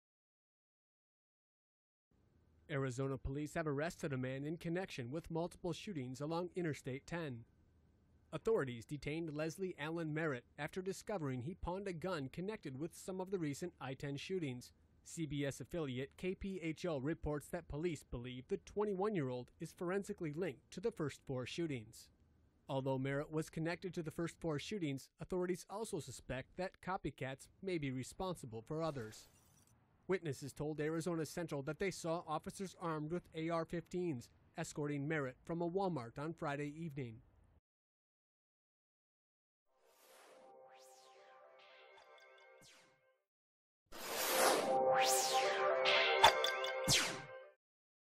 Arizona police detain suspect in Phoenix highway shootings
Originally published on 19 September, 2015

Arizona police have arrested a man in connection with multiple shootings along Interstate-10 in Phoenix.Authorities detained Leslie Allen Merritt after discovering he’d recently pawned a gun connected with some of the recent shootings. CBS-affiliate KPHO reports that police believe the 21-year-old was forensically linked the first four shootings.Although authorities believe Merritt was connected to the first four shootings, they also suspect that copycats may have been responsible for the other shootings along I-10. Witnesses told Arizona Central that they saw officers armed with AR-15s escorting Merritt from a Walmart Friday evening.Arizona Governor Doug Ducey broke the news on Twitter saying, “We got him”. Authorities had previously offered $50k for information leading to the arrest of anyone connected to the shootings.